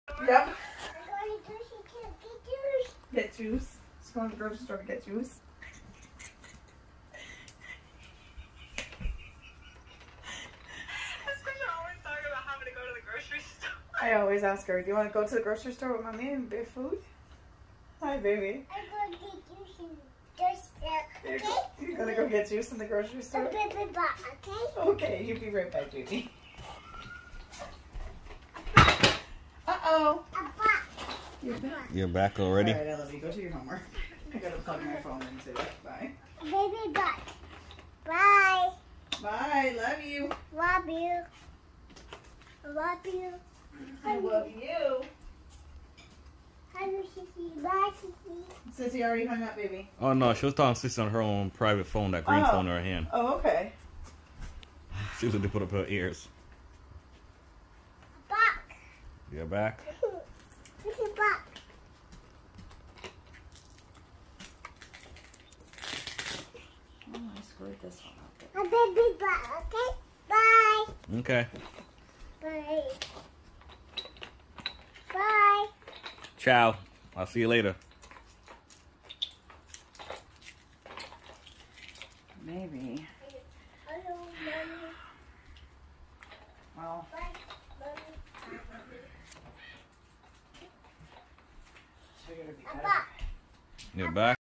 Going To The Grocery Store.
This video is about Hope pretending to go to the grocery store. She is doing that because her mom always asks her do you want to go to the grocery store. She then reached out to her sister Visa her own cell phone, to tell sister she's gone to the grocery store.